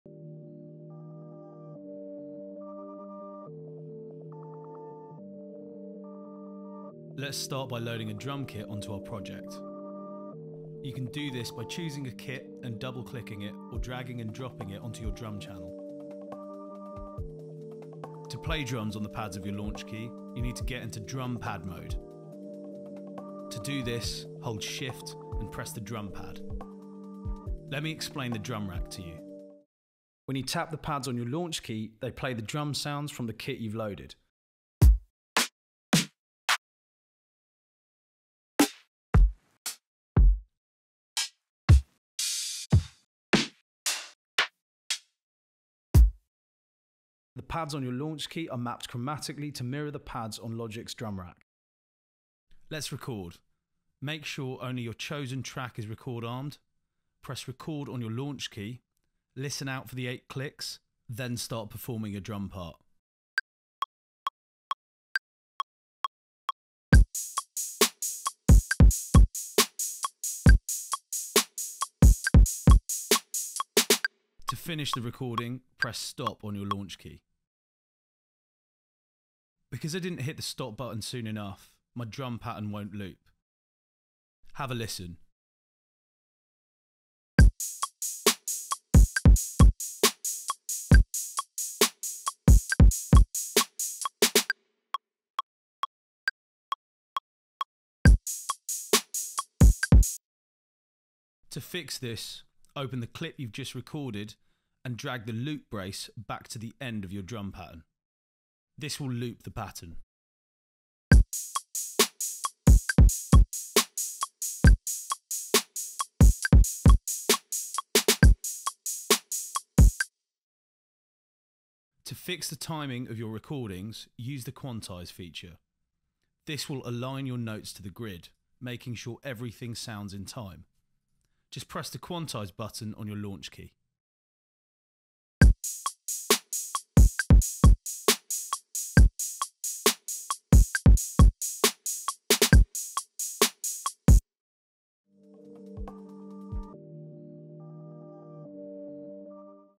Recording drums using Drum pad mode in Logic Pro
Chapters:

00:00 - Introduction
00:10 - Load Drum Kit
00:20 - Drum Pad Mode
00:30 - Drum Rack Explained
00:55 - Record Drums
01:42 - Loop Your Pattern
02:02 - Quantise Your Pattern